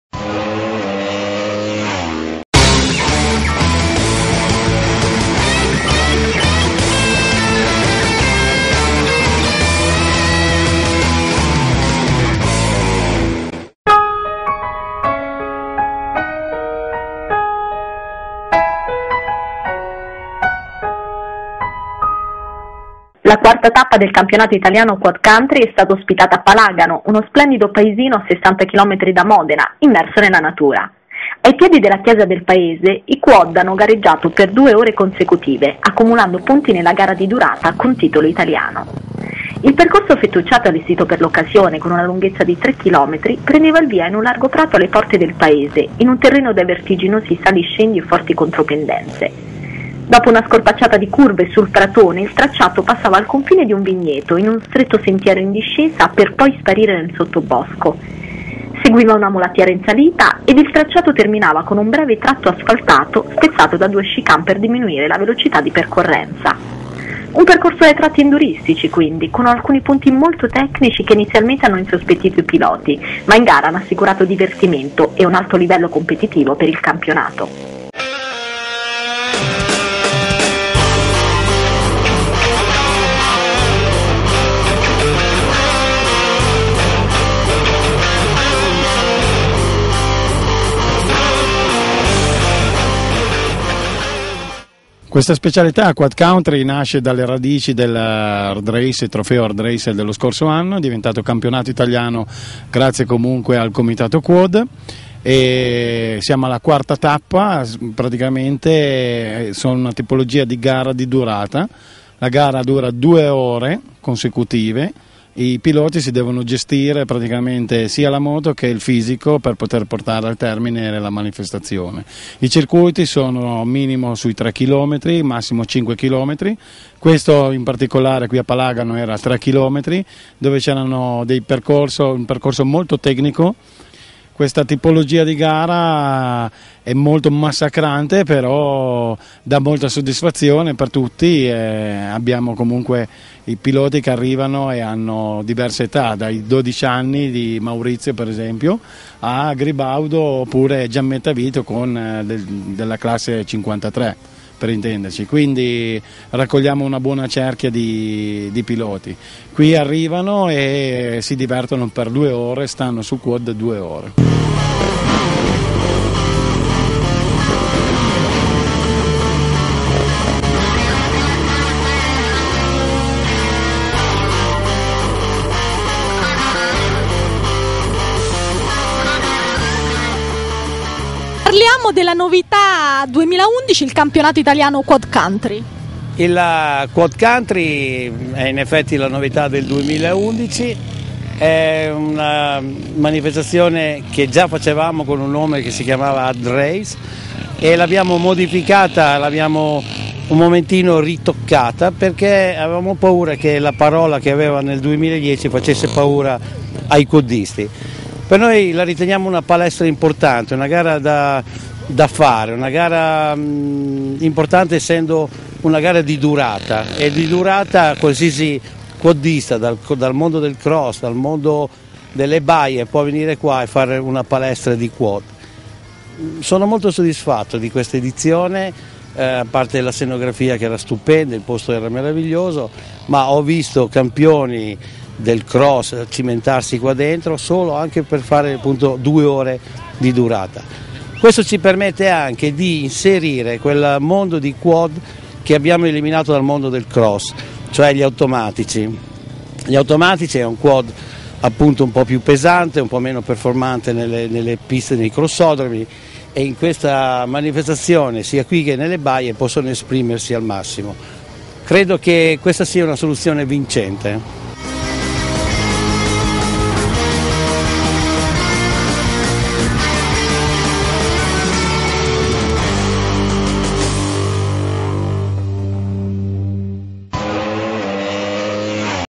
Alla scoperta del QuadCountry FMI 2011
In occasione della 4^ tappa del Campionato Italiano QuadCountry FMI 2011, che si è svolta a Palagano in provincia di Modena, il Comitato Quad FMI è andato alla scoperta di questa splendida disciplina, per raccontare con le immagini la gara di endurance per quad più bella d'Italia. Antonio Assirelli, Coordinatore del Comitato Quad FMI e Marco Magnetti, l'Organizzatore, raccontano le caratteristiche della specialità.